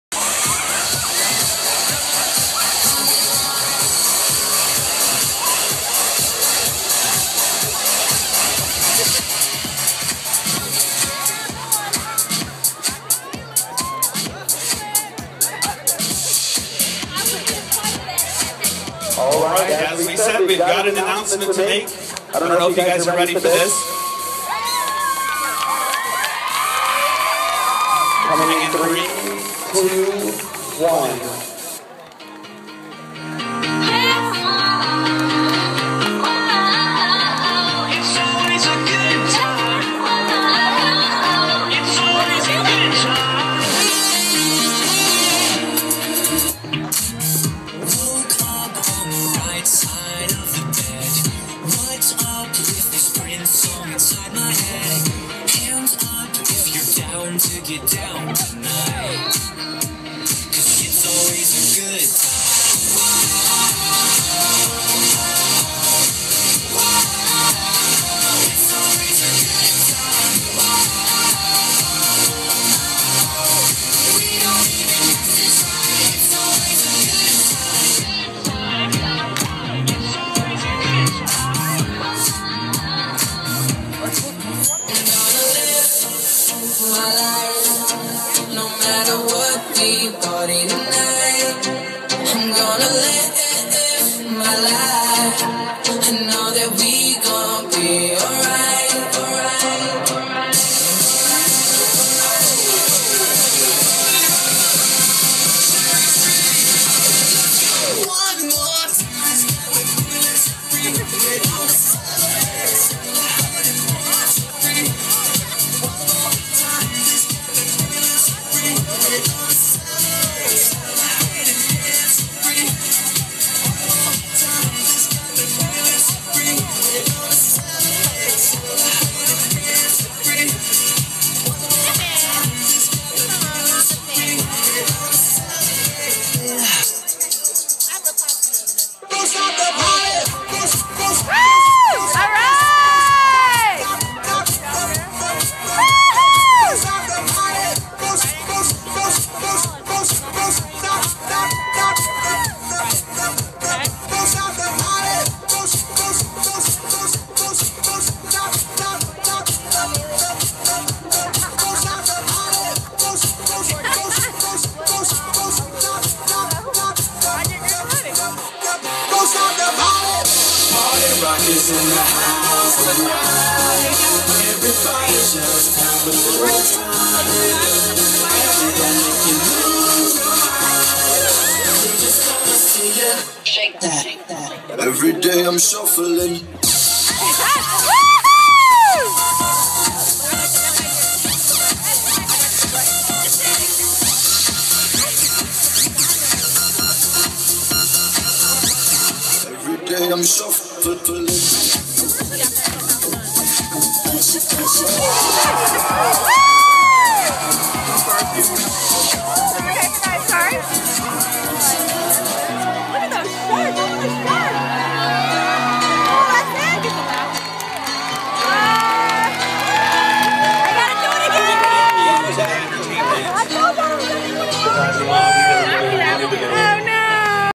Keesler Federal Credit Union Flash Mob, Relay for Life Event, May 4th 2013
KFCU's Flash Mob made another appearance at the American Cancer Society's Relay for Life Event at Biloxi High School Stadium. They were joined by a large group of Armed Forces Service members and others to make this the largest KFCU Flash Mob ever. Enjoy!